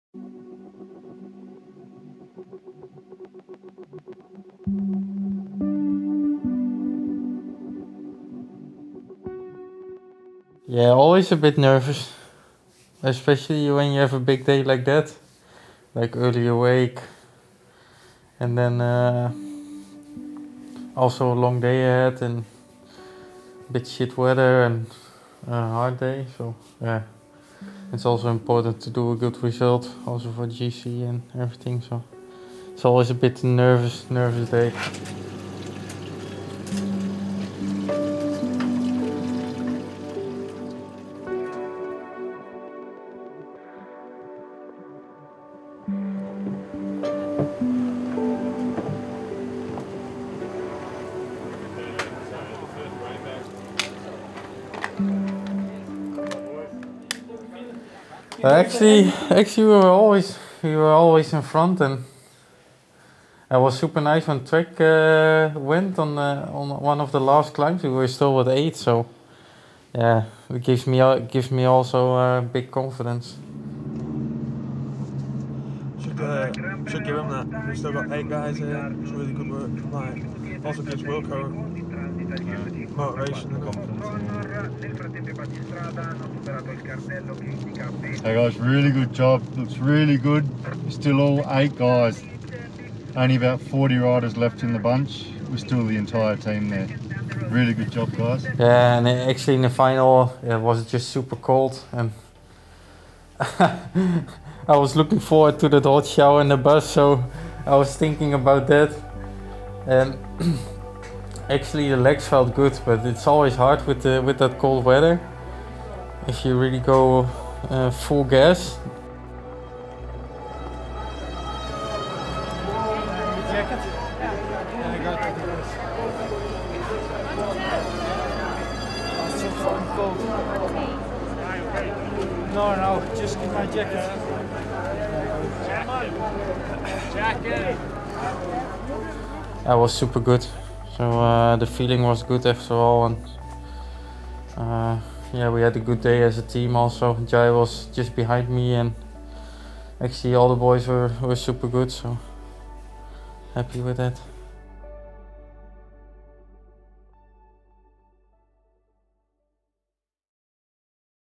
Into 2nd place | Giro d'Italia 2020 - Stage 9
On a tough, mountainous and rainy day at the Giro, Wilco Kelderman and Jai Hindley both finished inside the top 10 on the stage for us. Wilco now sits 2nd overall with Jai not too far behind in 9th as we head into the first rest day.